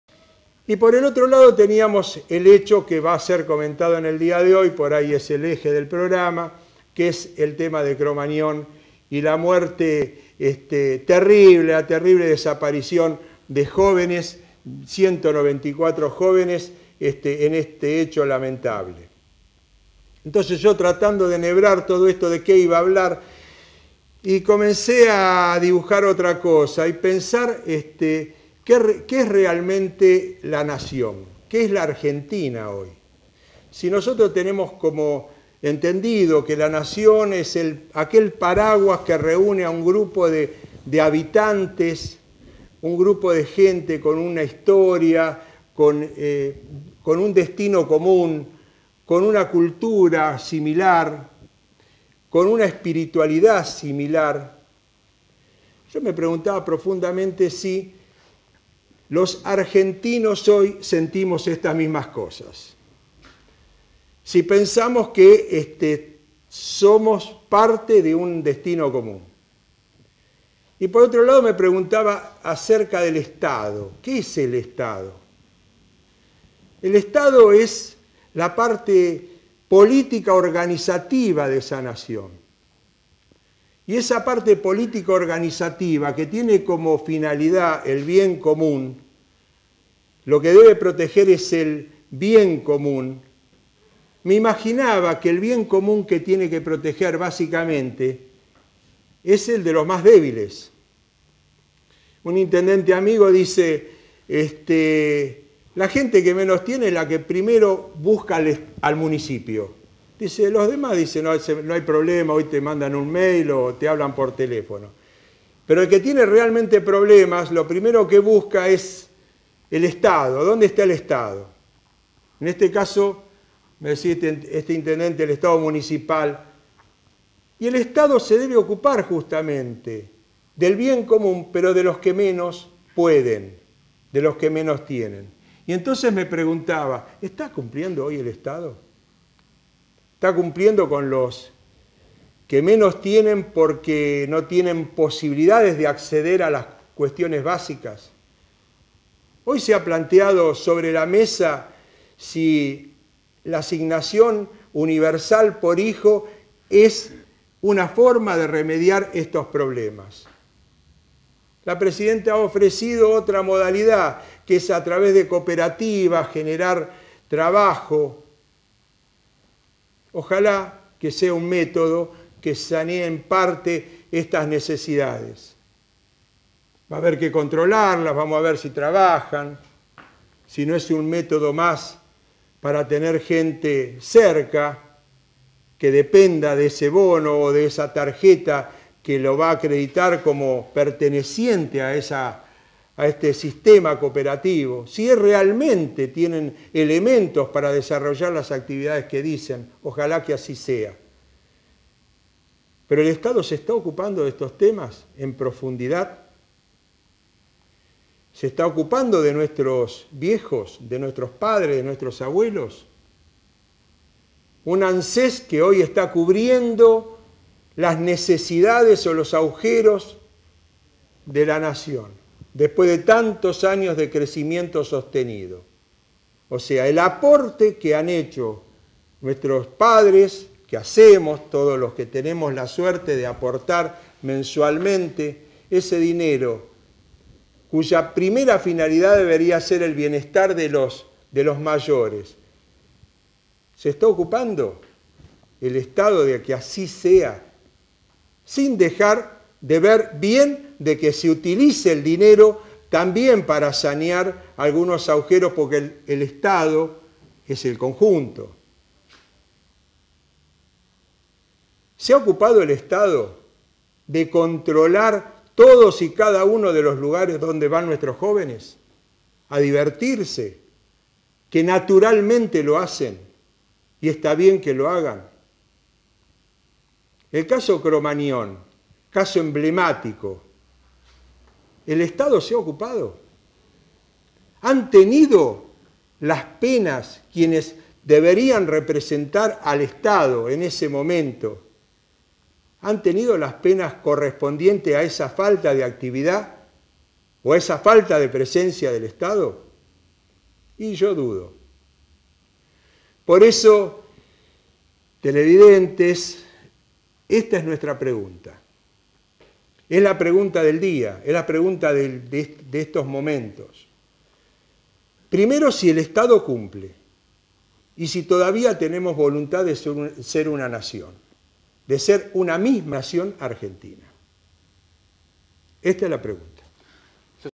DR. MIGUEL A. RIOS (programa 20/08/2009)
¿Se ocupa el estado de resolver los problemas estructurales de nuestra sociedad?